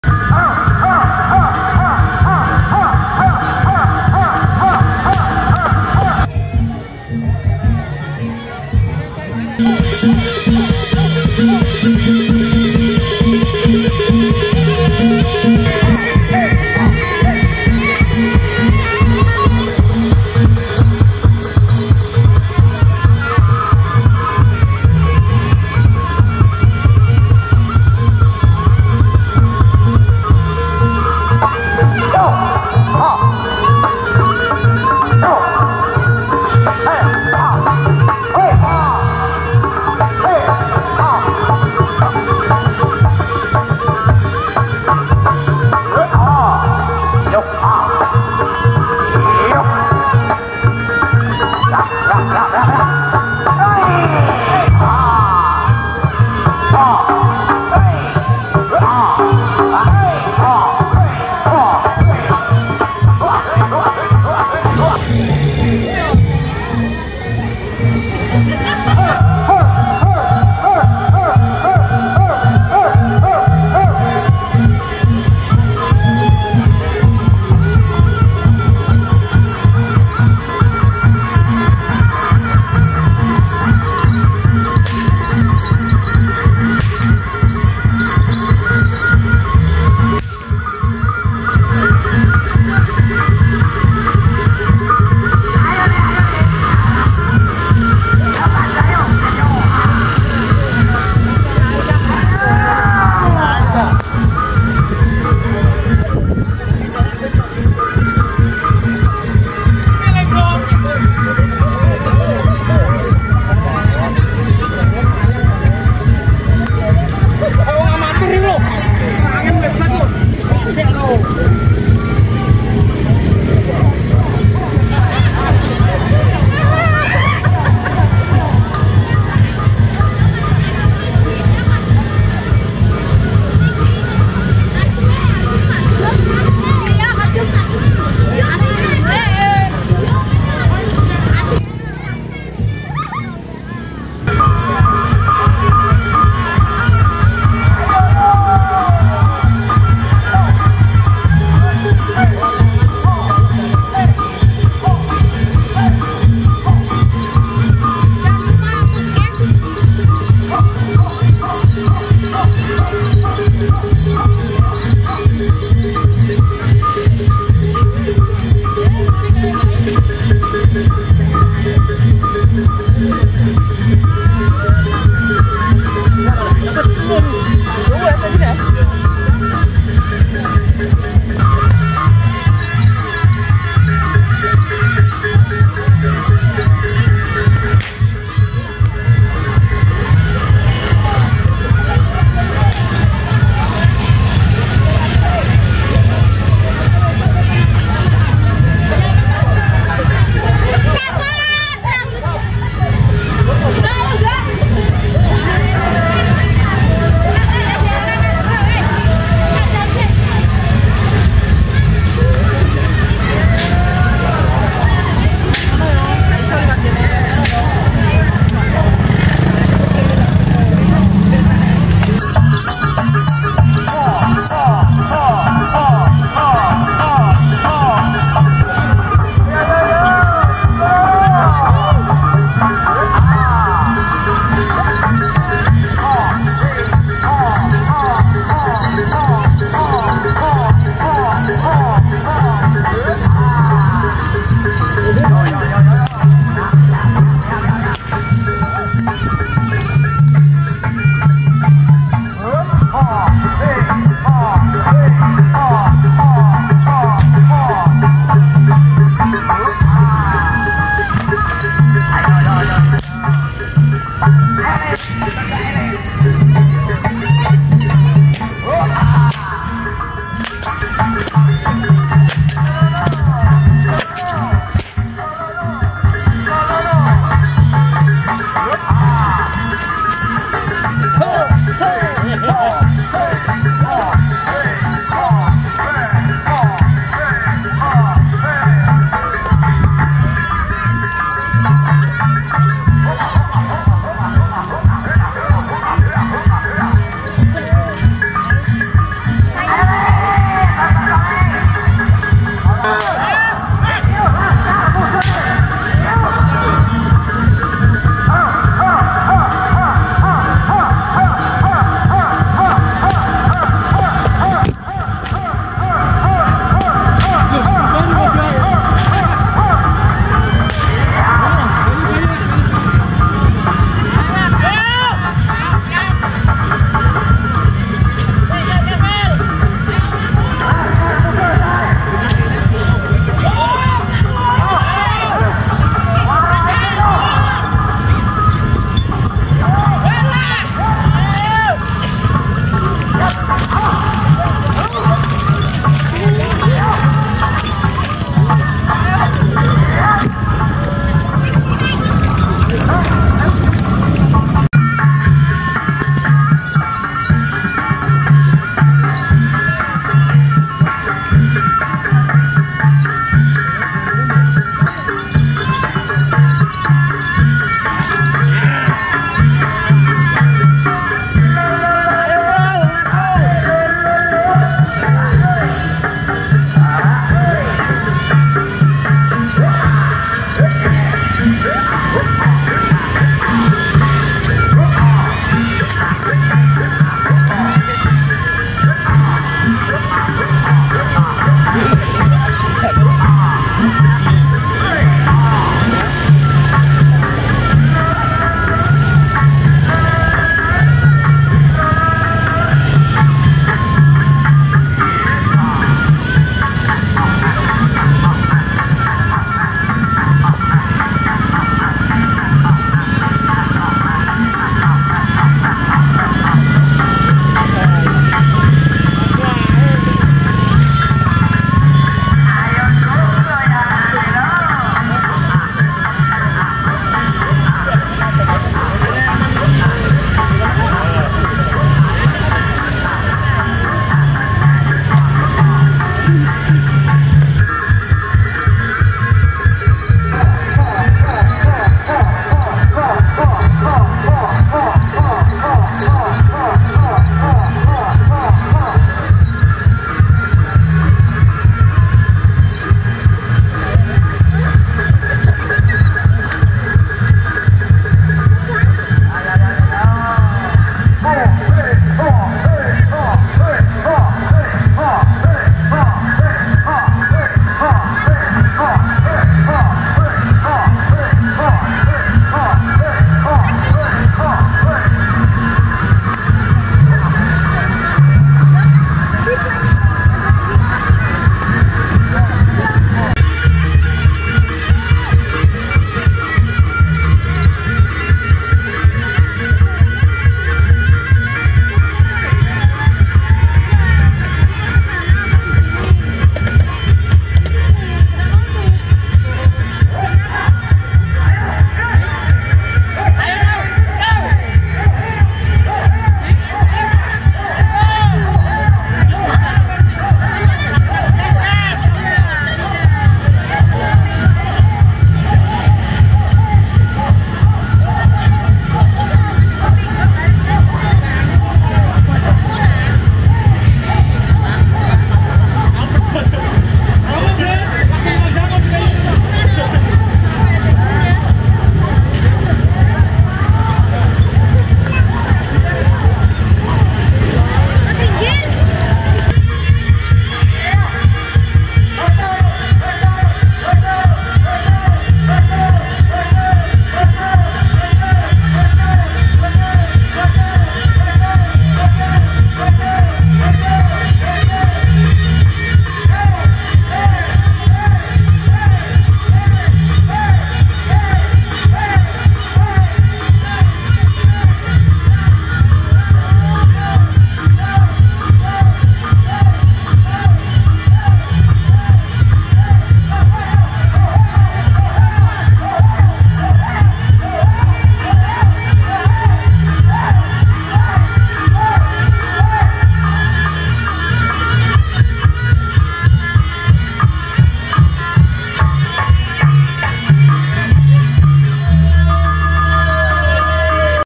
Karnaval Barongan &Reog Ponorogo LASKAR MUDHO Desa Wonosemi
Karnaval Reog Ponorogo dan barongan Blora Laskar Mudho Live Desa Wonosemi Banjarejo Blora